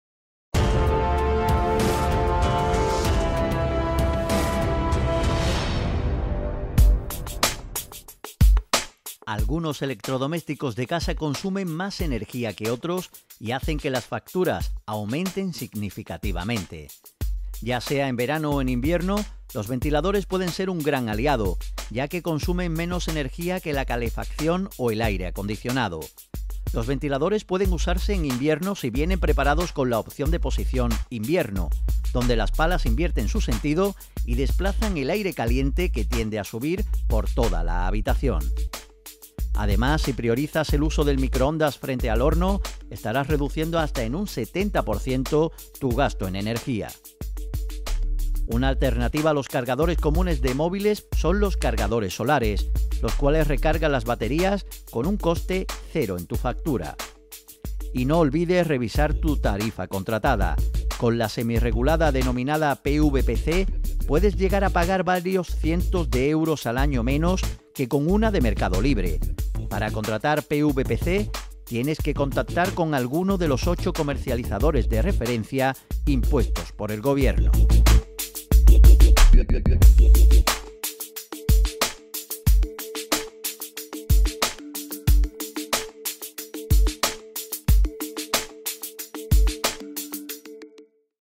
Aquí tienes tres alternativas para ahorrar en tu factura de la luz
Algunos electrodomésticos de casa consumen más energía que otros y hacen que las facturas aumenten significativamente.

Ya sea en verano o en invierno, los ventiladores pueden ser un gran aliado ya que consumen menos energía que la calefacción o el aire acondicionado.

Los ventiladores pueden usarse en invierno si vienen preparados con la opción de posición "invierno", donde las palas invierten su sentido y desplazan el aire caliente, que tiende a subir, por toda la habitación.

Además, si priorizas el uso del microondas frente al horno, estarás reduciendo hasta en un 70% tu gasto energía.

Por último, una alternativa a los cargadores comunes de móviles o portátiles son los cargadores solares, los cuales recargan las baterías con coste cero en tu factura.

Y no olvides revisar tu tarifa contratada. Con la semirregulada, denominada PVPC, puedes llegar a pagar varios cientos de euros al año menos que con una del mercado libre. Para contratar PVPC, tienes que contactar con uno de los 8 comercializadores de referencia impuestos por el Gobierno.

Este vídeo forma parte de una campaña de FACUA Andalucía financiada por la Dirección General de Consumo de la Consejería de Salud.